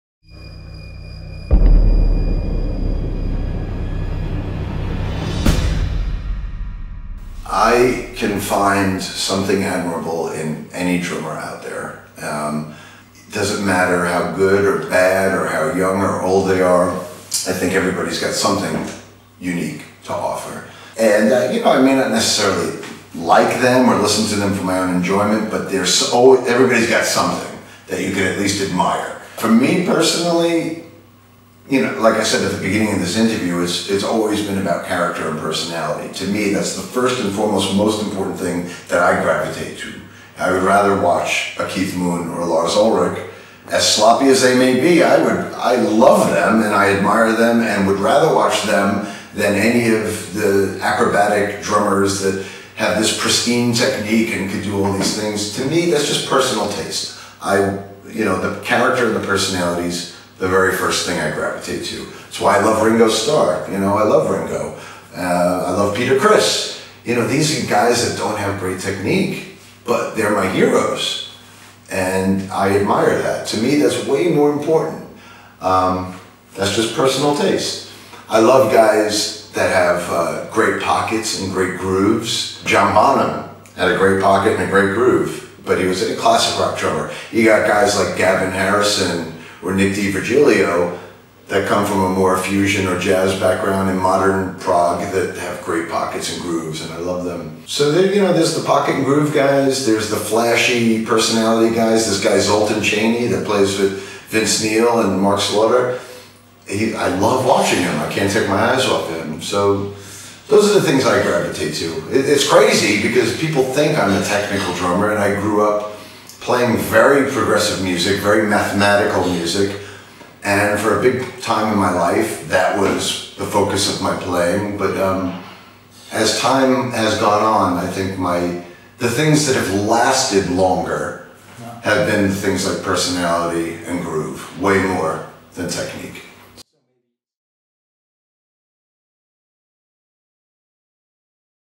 Mike Portnoy on other drummers - drumtalk [single strokes 08]
Of course technique and skills on the drumset are a significant expansion for your vocabulary. But what about character and personality? Mike Portnoy definitely has a point here!